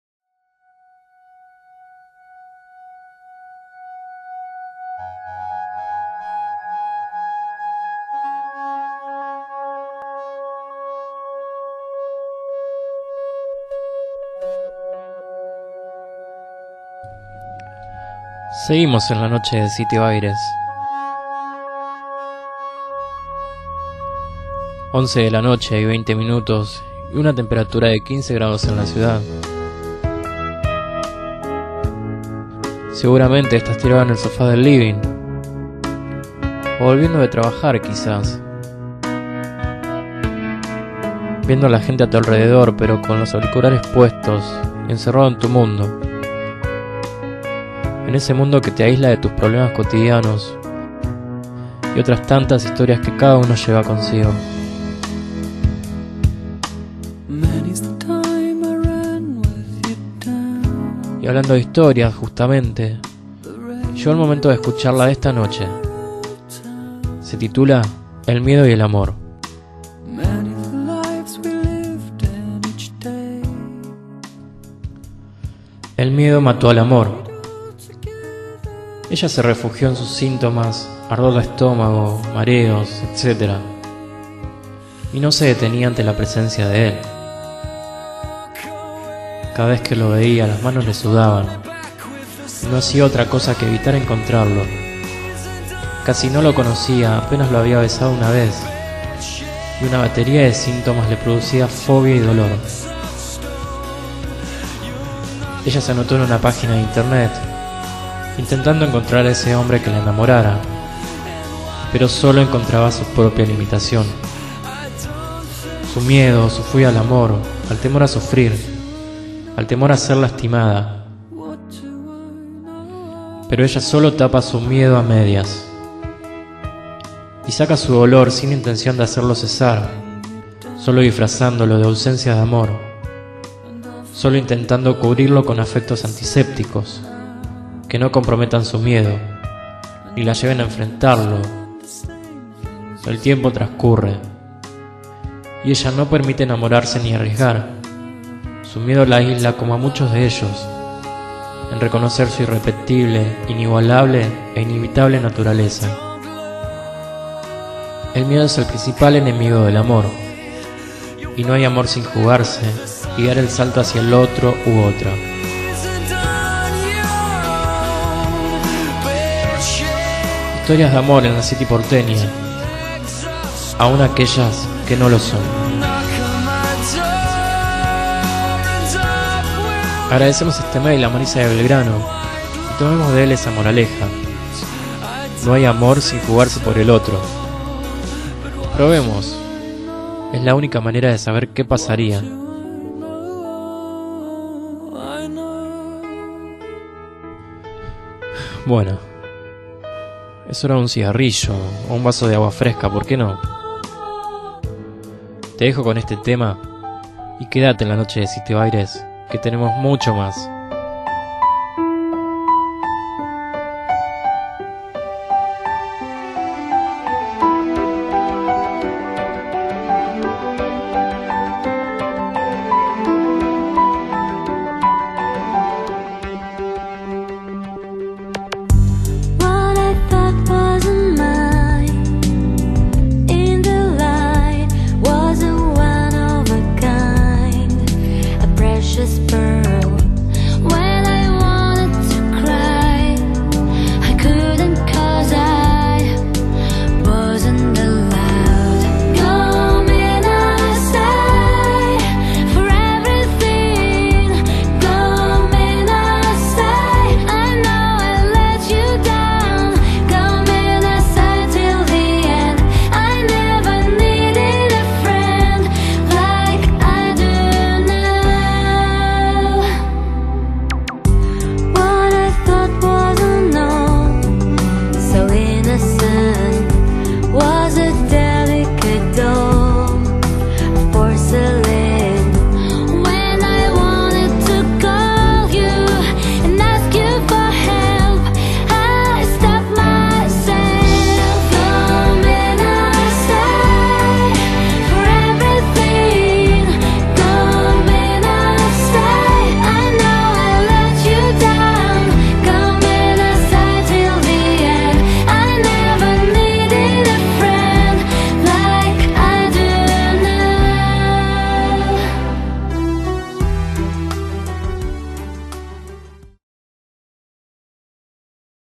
Historia de amor by Sergito •°o.O Autentic Boy O.o°•
Uno de mis tantos trabajos en radio... una de las tantas historias de amor que encontramos en la ciudad... espero lo disfruten